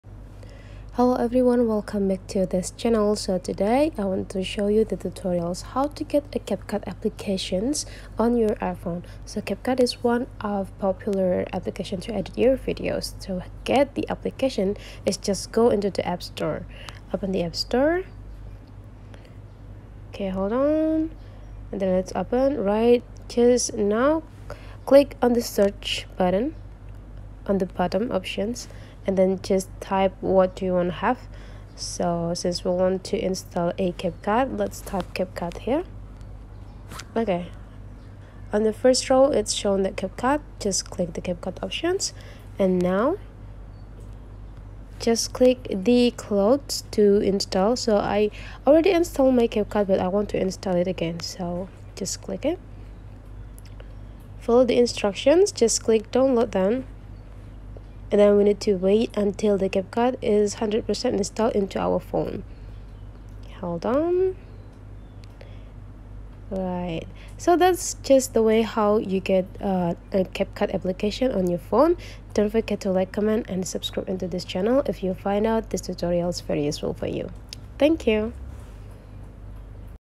Wondering What Applications to Edit Videos? CapCut May a Best Choice to Have, Get Your CapCut Now
CapCut is a popular application to edit the video. CapCut is completed with many features that easy to use. Here is how to get CapCut on your iPhone